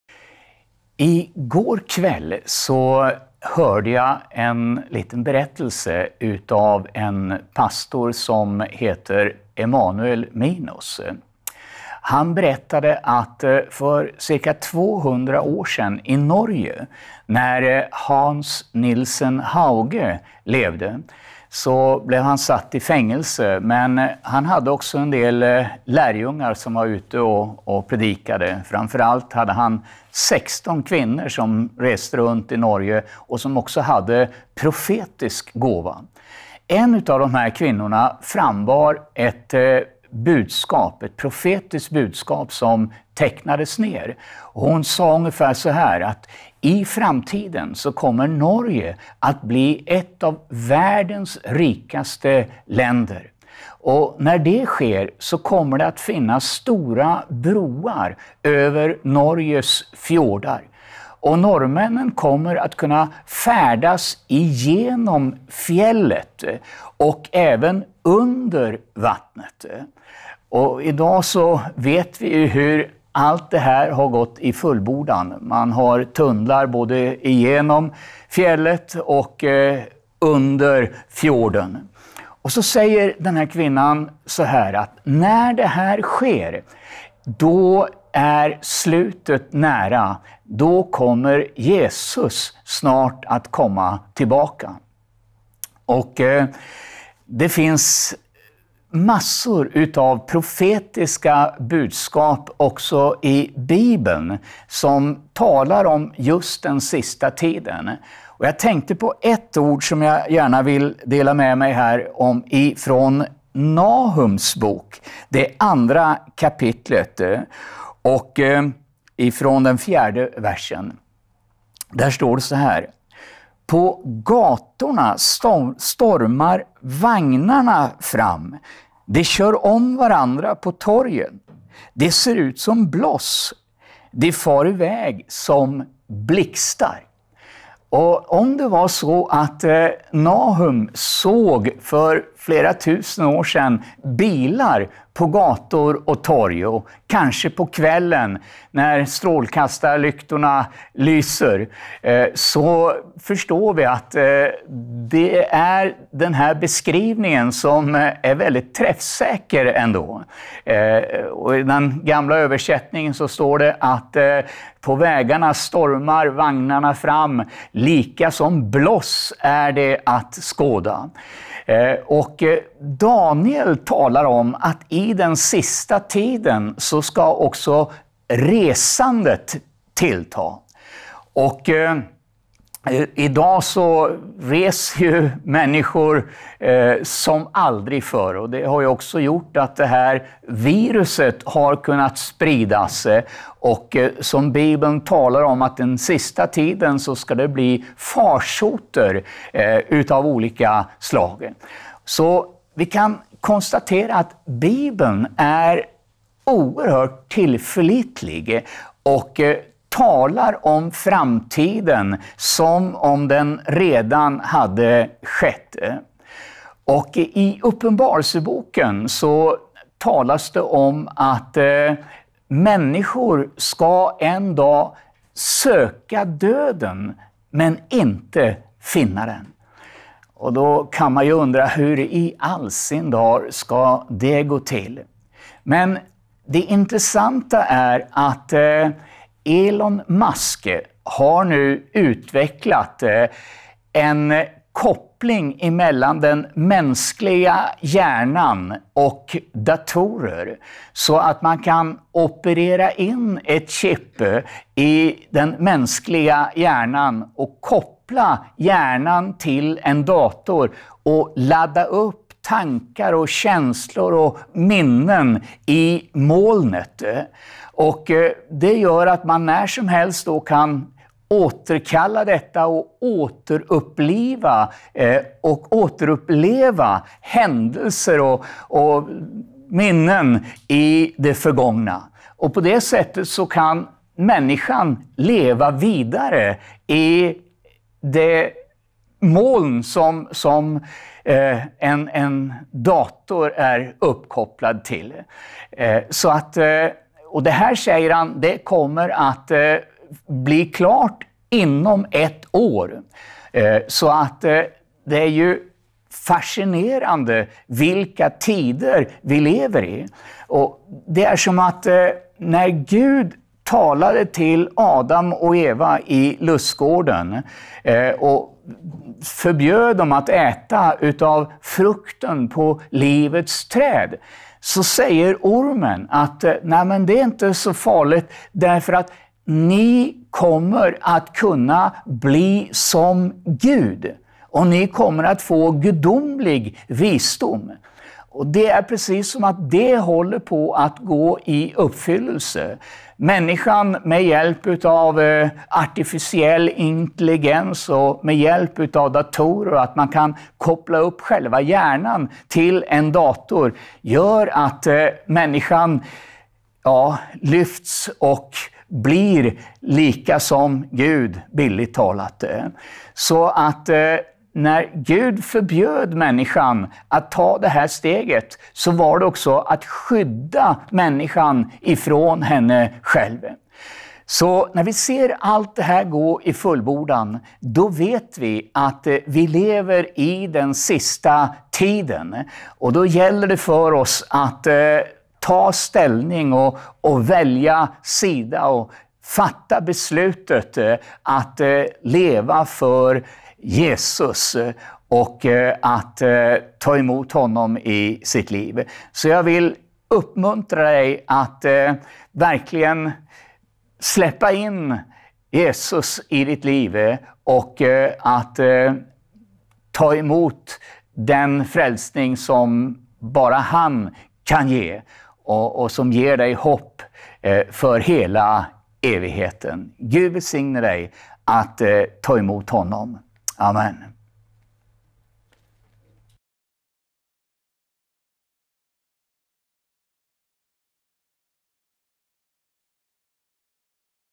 Profetiorna har gått i uppfyllelse framför våra ögon | Ove Marcelind
Är det billampor profeten Nahum beskriver i Bibeln? Och hur kunde en kvinna veta så långt förväg hur Norges infrastruktur skulle se ut idag? Ove Marcelind tolkar och förklarar några intressanta profetior om Jesu tillkommelse.